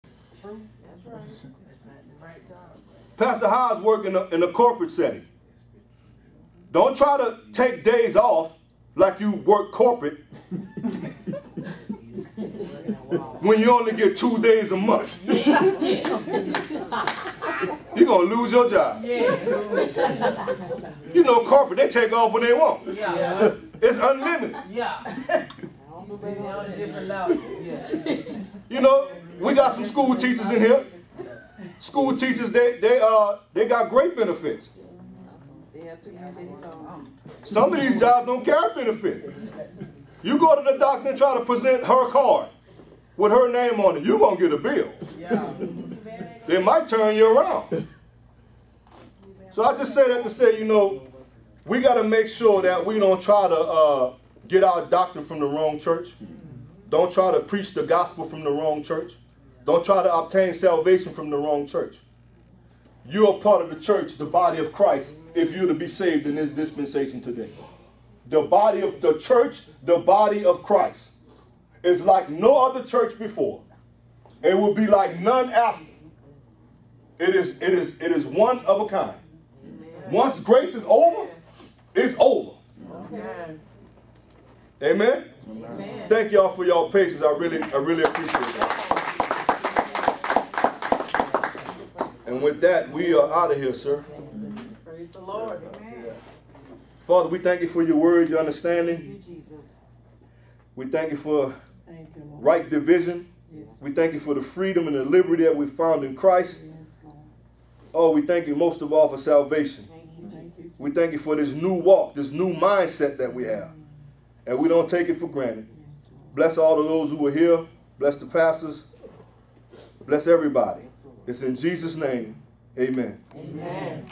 What church do you attend? Part 2
What church do you attend? Is it the church in the wilderness? The church in the temple on the day of pentecost? Or is it the church, the body of Christ?